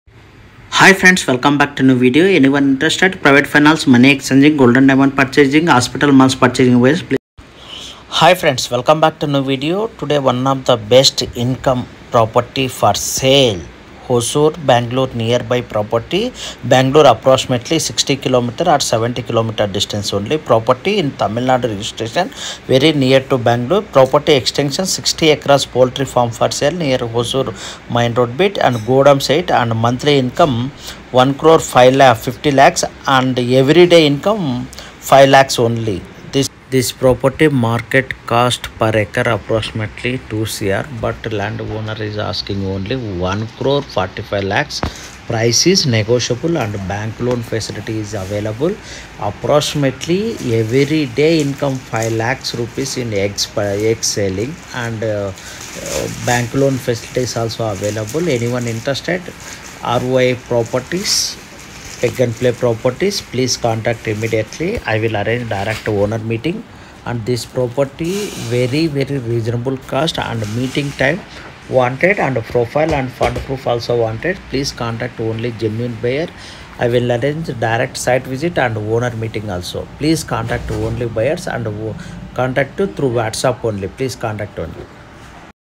Every day income- 5Lakh || 60 Acres poultry farm sale
LOCATION: VILLAGE, TAMIL NADU (about 15 kms from Hosur)
LAND EXTENT: 60 ACRES & more.
DETAILS OF THE PROPERTY:
Layer Poultry Farm with farm capacity of about 300000 birds (Two Lakhs Eighty Thousand Birds)
Facility includes (MEASUREMENTS GIVEN SEPARATELY)
• 2 No's Brooding sheds with gas brooding equipments.
• 2 No's Grower Sheds.
• 6 No's Layer Sheds.
• Feed Mill with HARDCASE ENGINERING FEED MILLS 2 No's with 6 tons/hr and 3 tons/hr crushing capacity.
• Feed Mill Storage area attached to the feed grinding area.
• Feed Storage warehouse 4 nos.
• Staff Quarters.
• Labour Quarters.
• Staff Quarters Near brooding Area.
• 50 tons ESSAE weigh Bridge.
• Water Ozonization Treatment Plant.
• Security Rooms.
• Egg Storage Go down.
• Stores Area 2 Nos.
• Staff Office.
• MD Office.
• Farm Guest House.
• Water Tank with Storage of water at the Sump Area(75000 litres) and Overhead Tank for water storage (75000 litres)
• Running Bore wells.
• About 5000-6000 trees of Teak, Silver Oak, Yabev (used for making furniture).
• 5 acres of coffee and pepper plantation.
• Separate water tank for storage of bore well water for agriculture purposes 40000 litres
capacity.
• Canteen Building.
• Arcanut Plantation ABOUT 2000 nos.
• Yielding Coconut, mango, Chikoo, Lemon, Fig and Jack Fruit Trees.
• Area fully fenced with barbed wires and stone support pillars.
Generator room with 2 no's generators, 63KVA ×1, 125 KVA x1, Stabilizer 1nos of 100 KVA